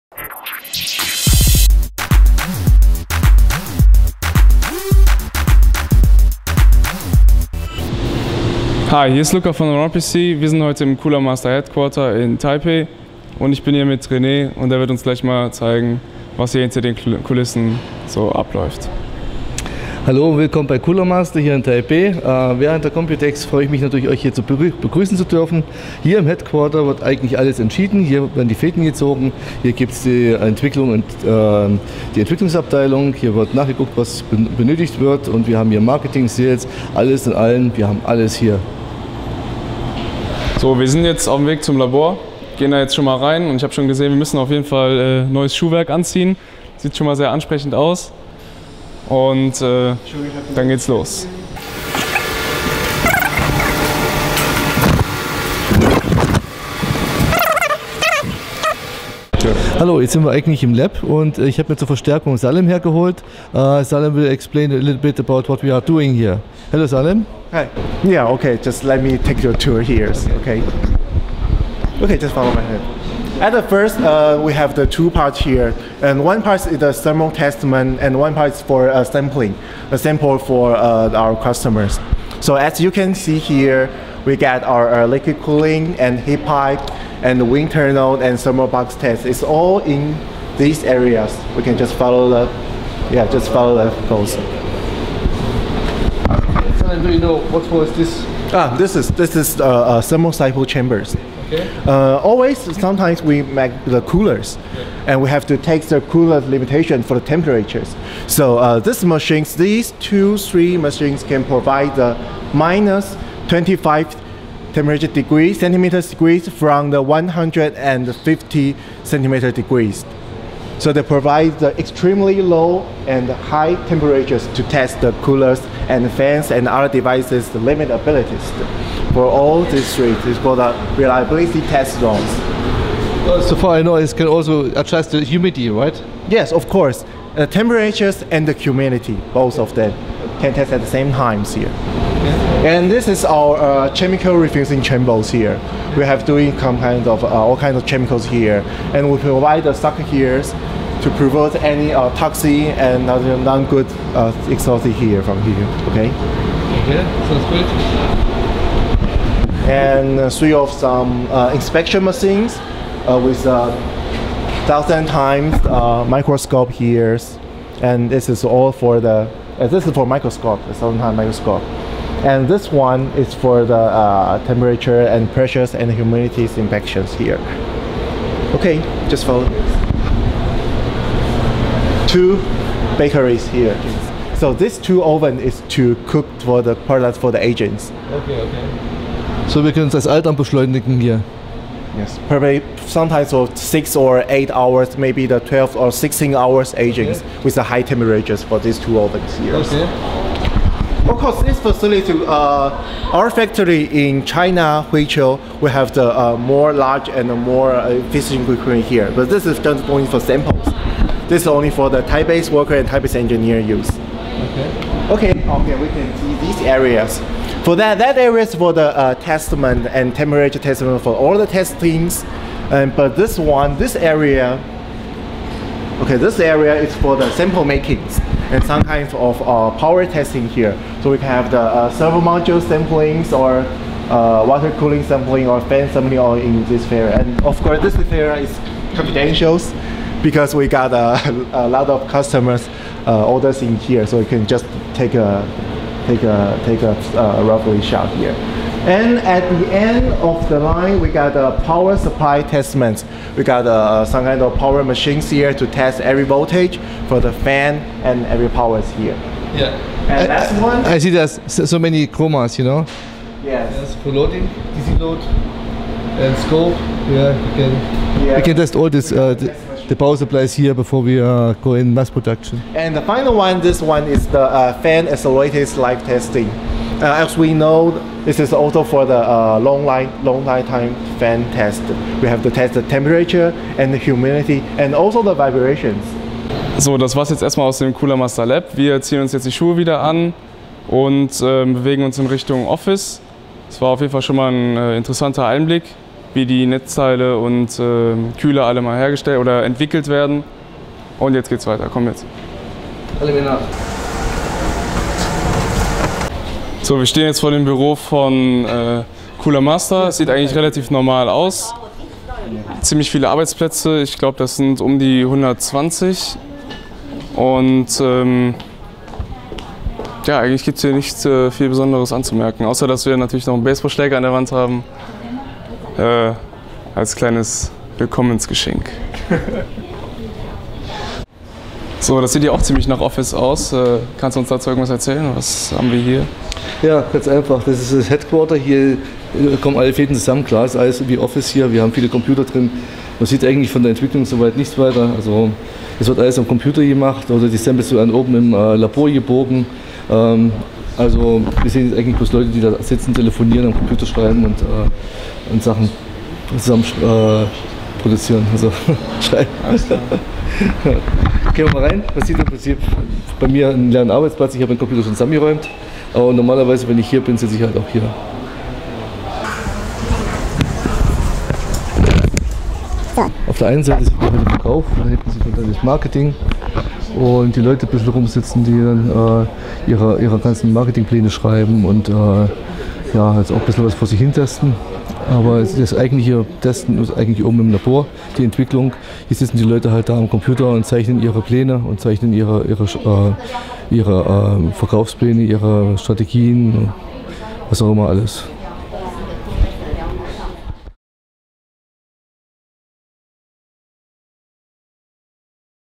Computex: Cooler Master HQ-Tour in Taipei
Auf der diesjährigen Computex waren wir natürlich auch außerhalb der Messe in der taiwanesischen Hauptstadt Taipei unterwegs und haben unter Anderem das Headquarter von Cooler Master besucht. Viele spannende Eindrücke und einen Blick in die tägliche Arbeit der Mitarbeiter verschaft euch dieses Video!